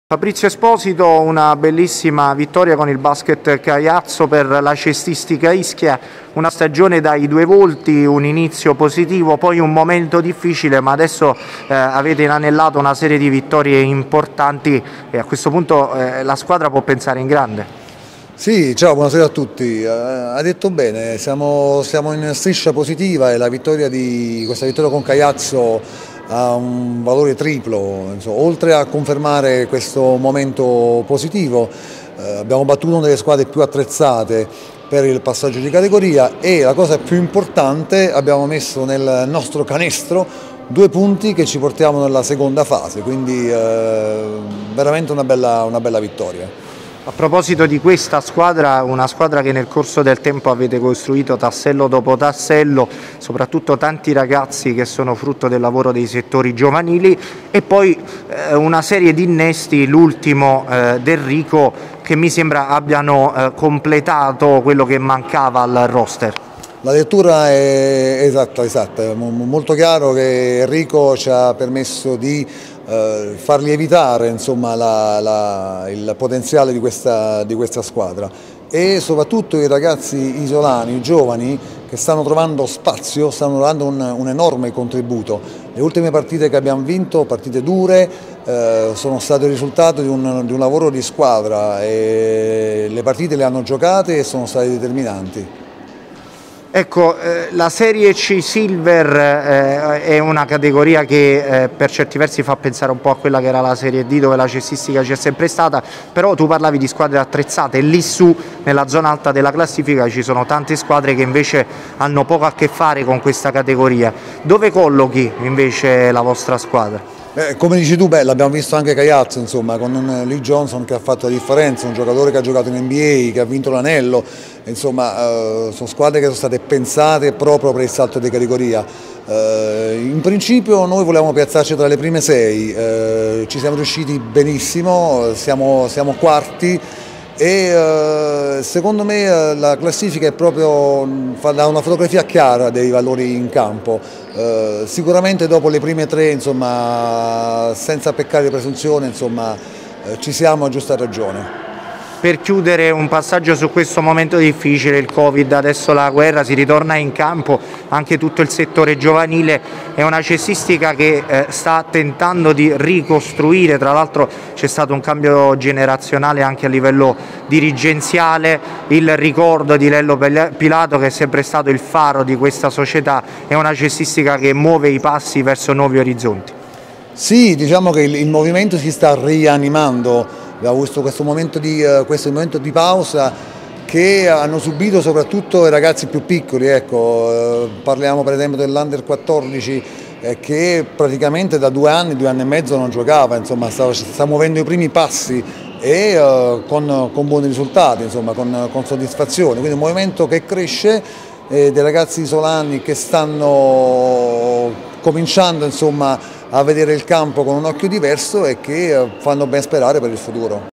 BASKET – LA CESTISTICA ISCHIA CONTINUA A VINCERE E SOGNA LA C GOLD.
BASKET – LA CESTISTICA ISCHIA CONTINUA A VINCERE E SOGNA LA C GOLD. FABRIZZIO ESPOSITO: “LA SQUADRA HA TROVATO IL SUO EQULIBRIO, ORA NON CI PONIAMO LIMITI”

Con la vittoria casalinga ottenuta sabato scorso contro il Caiazzo per 84 a 73, la Cestistica Ischia in serie C silver ha conquistato la quinta vittoria consecutiva contro, tra l’altro, una delle squadre favorite alla promozione nella categoria superiore. Soprattutto dopo l’arrivo di Errico, la squadra di coach Russelli e di coach Esposito ha ritrovato entusiasmo ed equilibrio ma soprattutto punti che permettono agli isolani di accendere alla seconda fase del girone promozione in cui ci si potrà giocare la promozione in serie C Gold. Un obiettivo ambizioso, probabilmente più un sogno che un obiettivo raggiungibile, ma la Cestistica Ischia del presidente D’Abundo non intende porsi limiti.